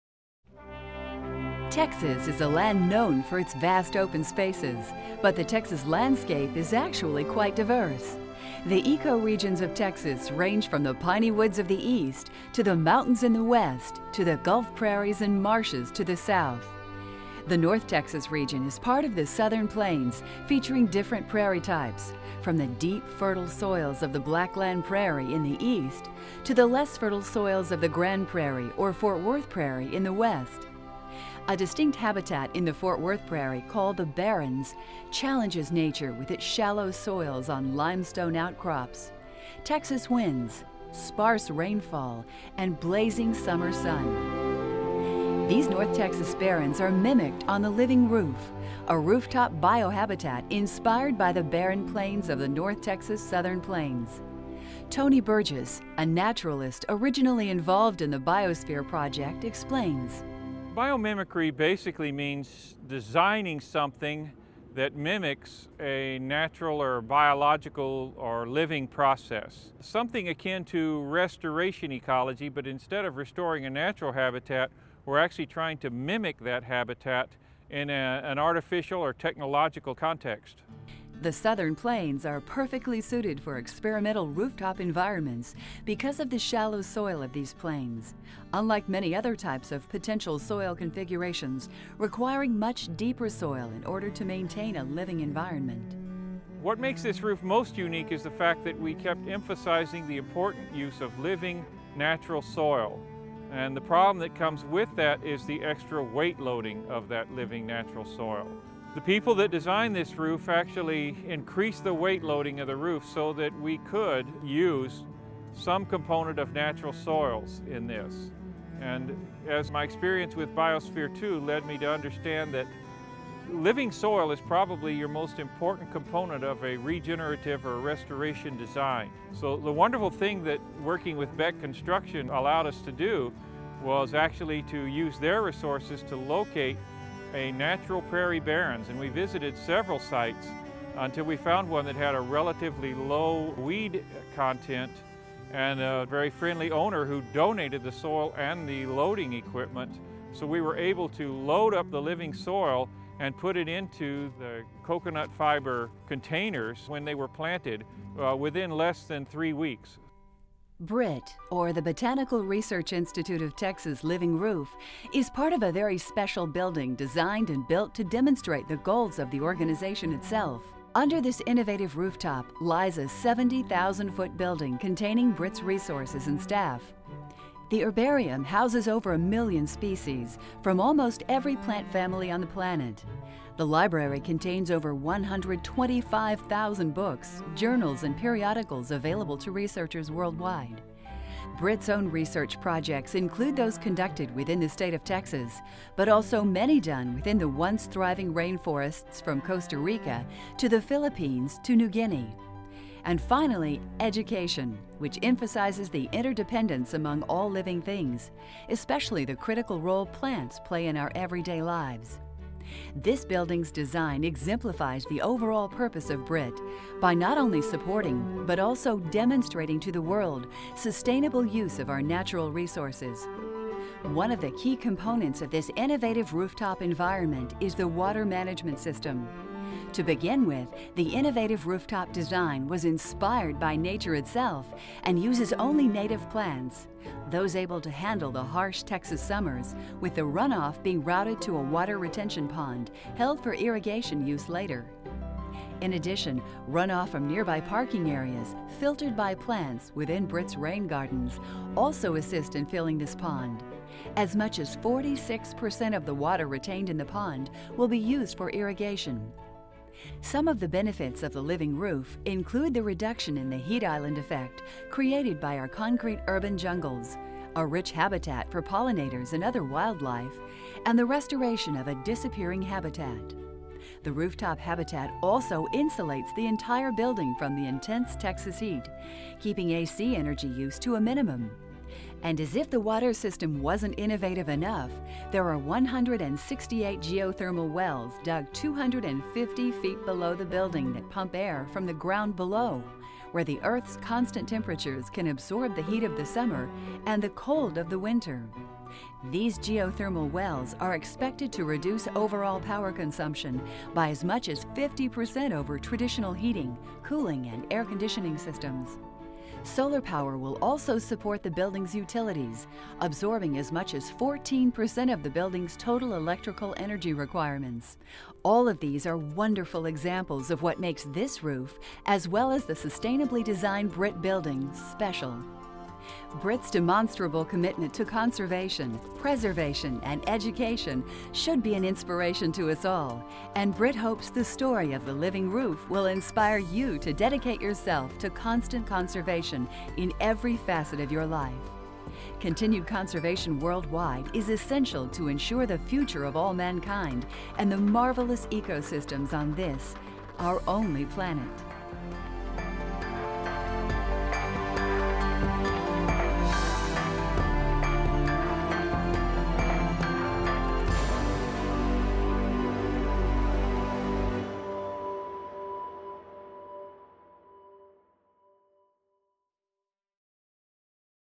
The Living Roof at BRIT
Did you know that the Botanical Research Institute of Texas has its very own living roof?!  Check out this video to learn more about the research and background of the living roof designed for BRIT's new campus that was established in 2011.